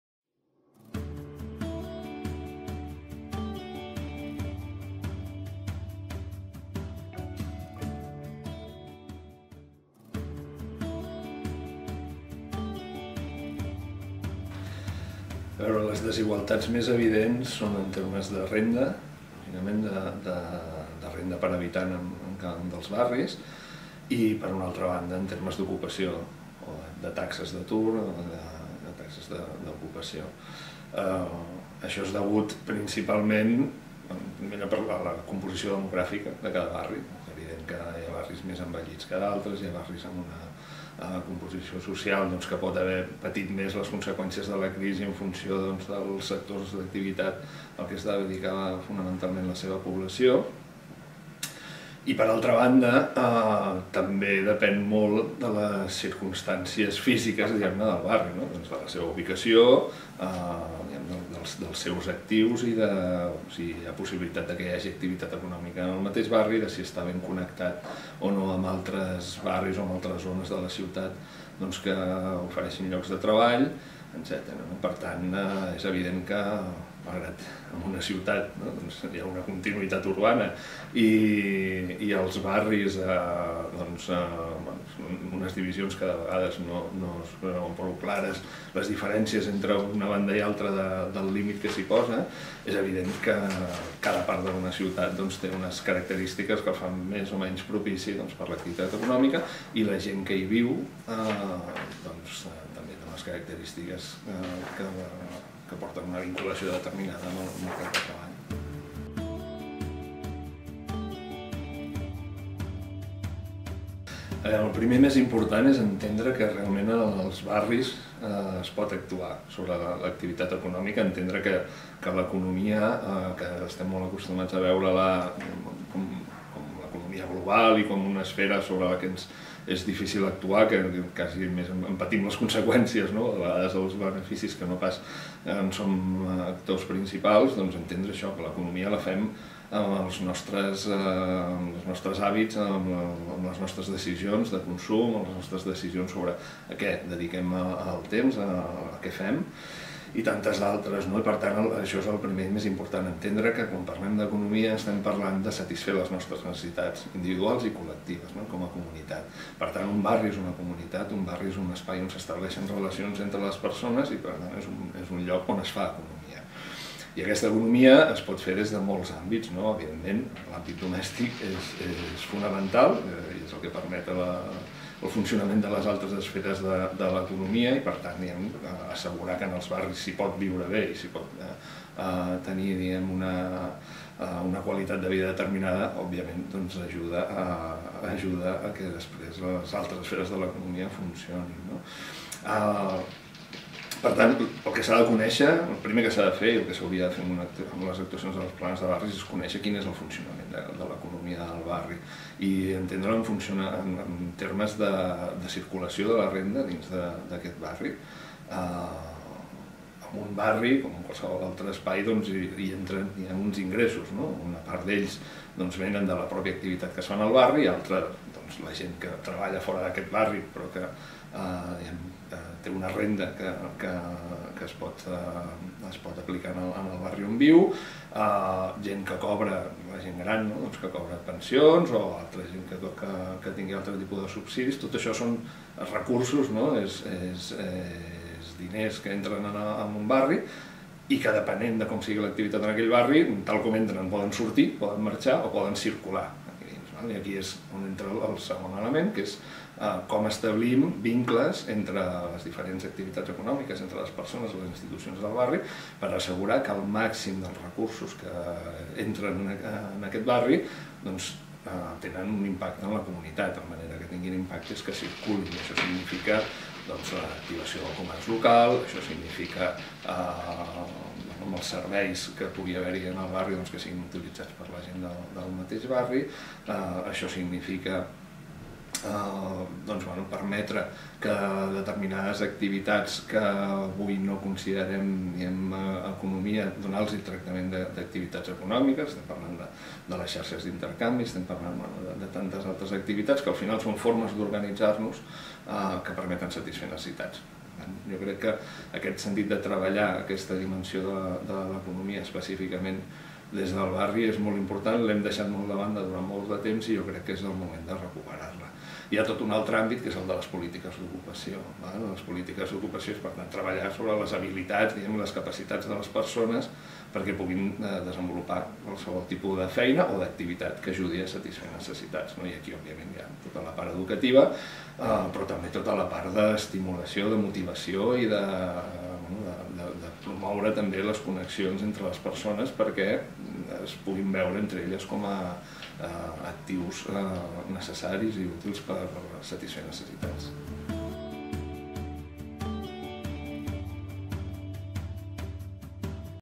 El Consell Assessor del Pla de Barris: Oriol Estela (Activitat econòmica)
"Transformar la ciutat amb la ciutadania: Criteris i reflexions per al Pla de Barris de Barcelona. Consell Assessor del Pla de Barris". Oriol Estela (Activitat econòmica)